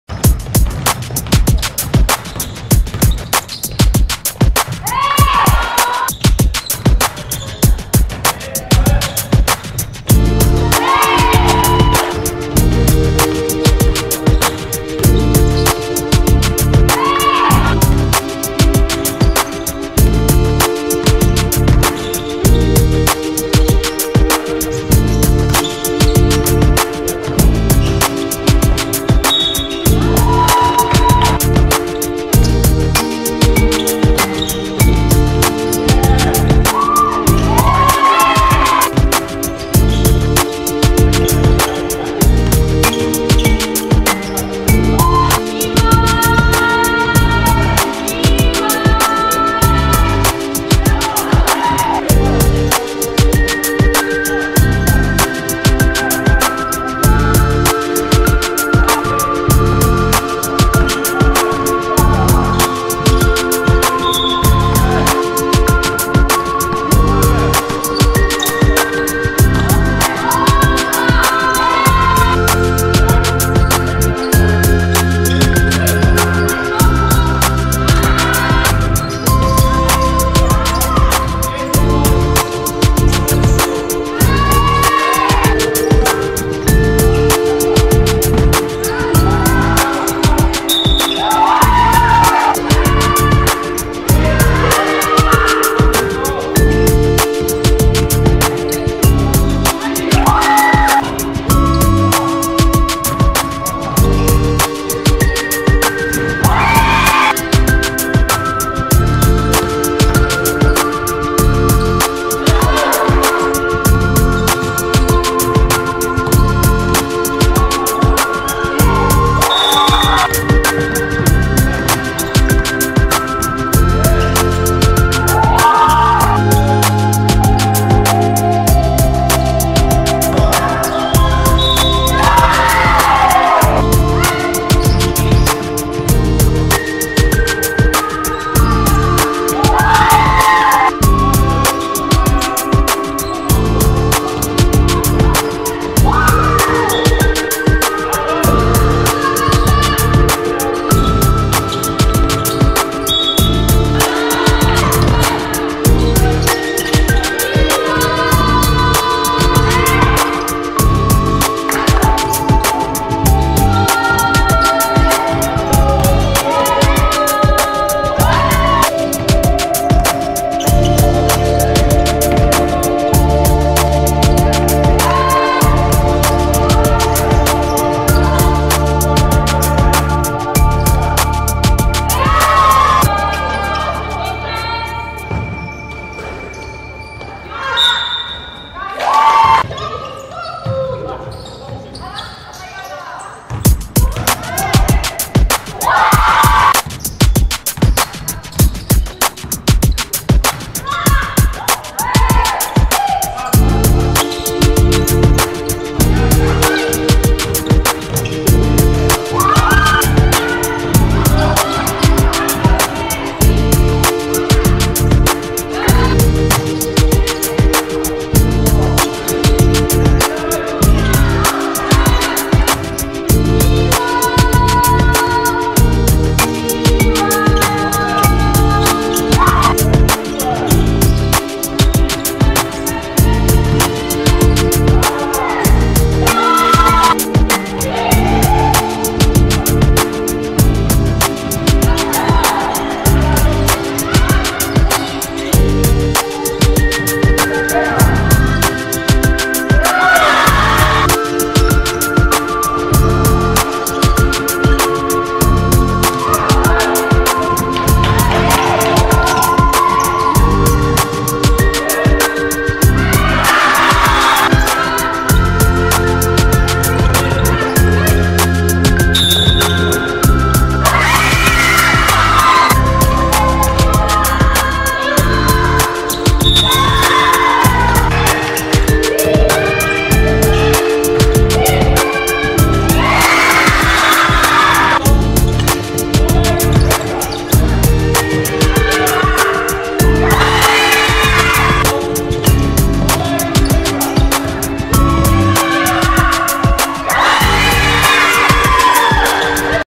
05 08 Los Angeles vs Golden State – DC81 Girls Zambrów 2022
LOS ANGELES 79:77 GOLDEN STATE po dogrywce
D.Starczewska 24pkt 10zb 3ast
A.Maławy 21pkt 15zb